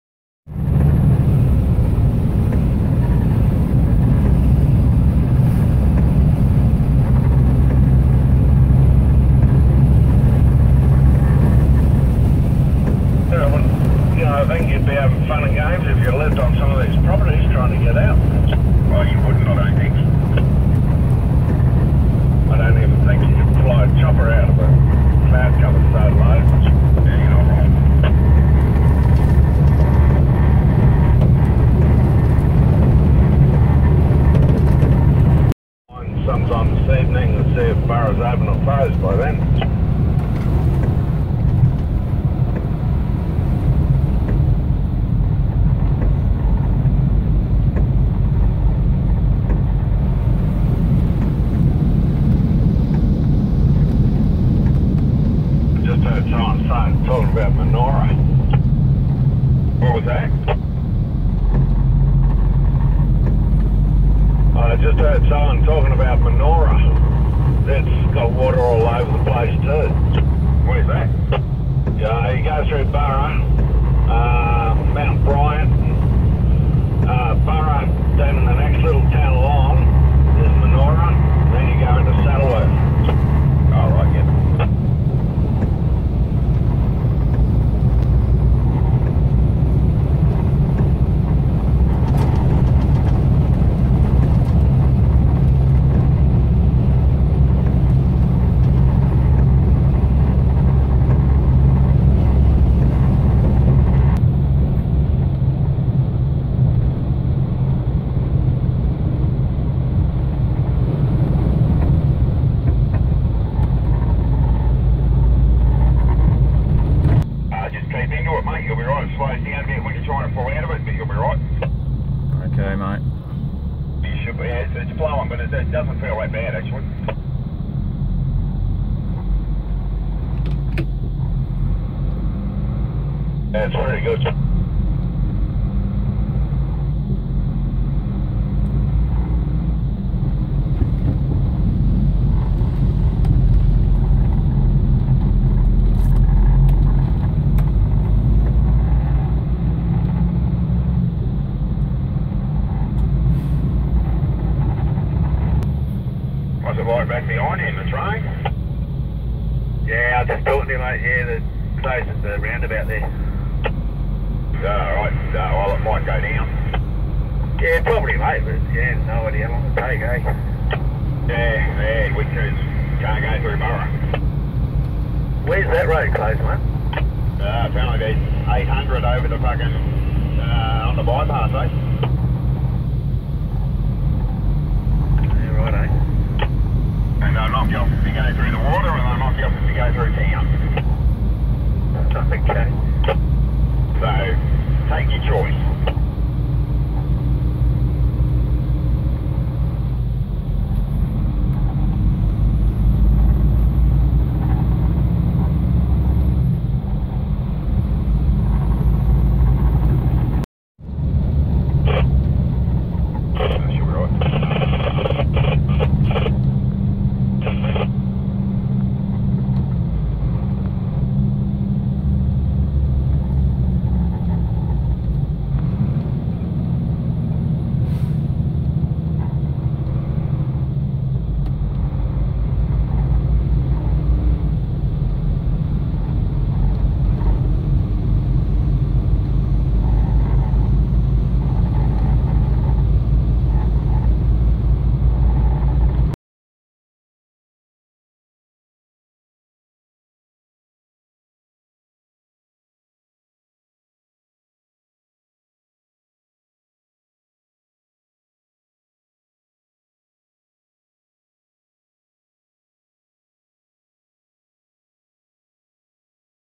Trucking Flooded Roads | Australia
A short video taken in South Australia during the heavy rains and flooding the state endured in late september 2016. These roads are through the mid north near the towns of PeterBorough , Jamestown and Gladstone.